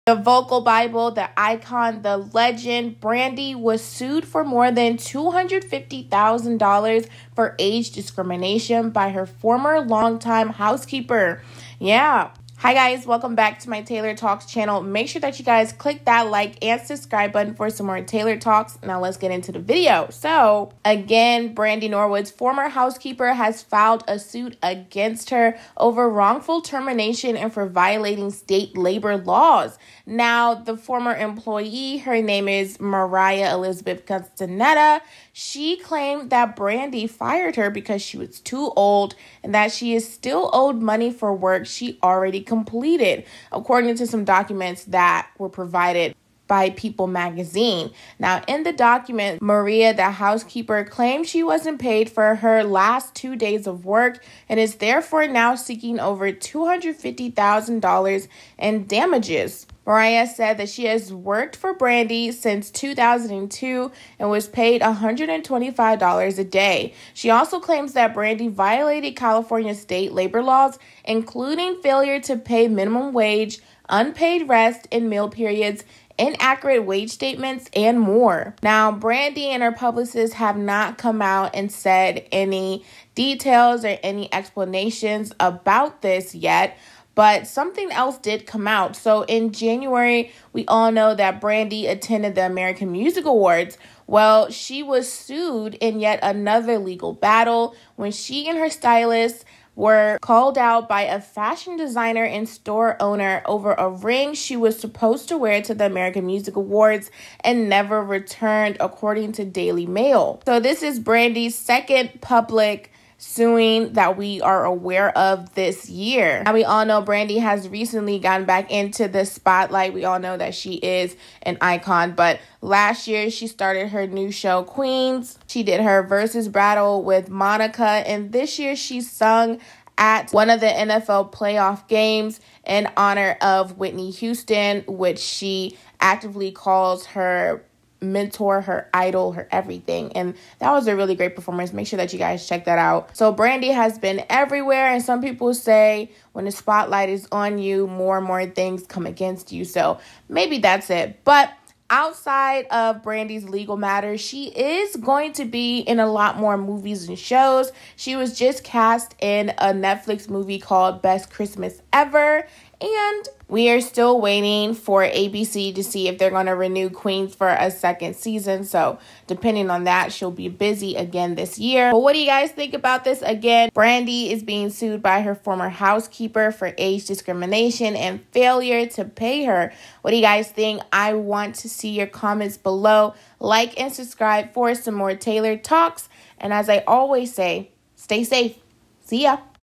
Brandy’s Housekeeper Sues For $250,000 Alleges Age Discrimination And Failure To Pay
Brandy, The Vocal Bible is being sued by her former housekeeper of over 20 years! Her housekeeper Maria Elizabeth Castenada claims she wasn’t paid for the last two days of work, and is seeking over $250,000 in damages. Maria claimed that she was fired over age discrimination and that Brandy wanted to hire a younger housekeeper.